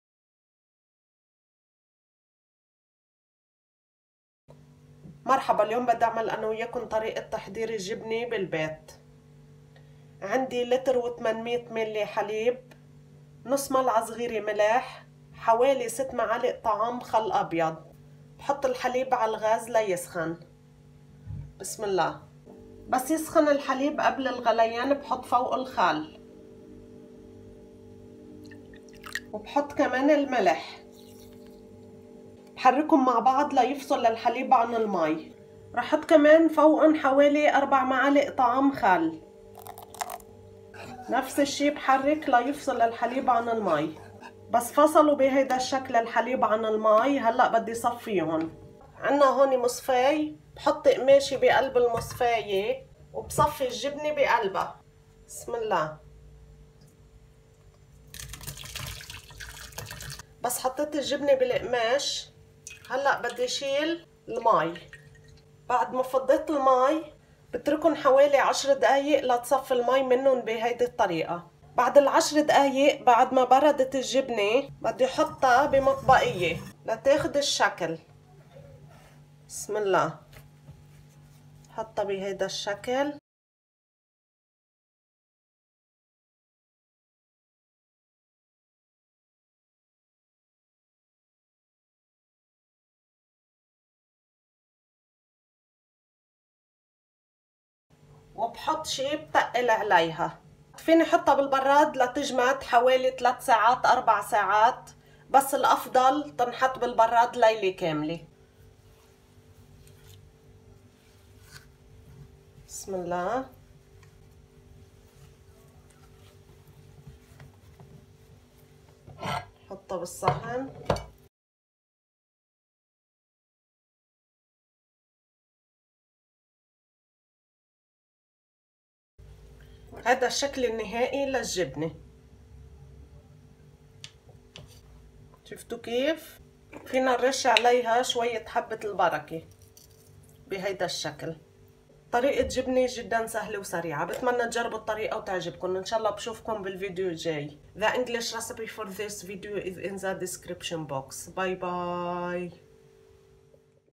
Easy Cheese-عمل الجبنة في المنزل بكل سهولة بدون منفحه
Ingredients
1800 ml of milk
1/2 teaspoon of salt
10 table spoons of white vinegar
black cumin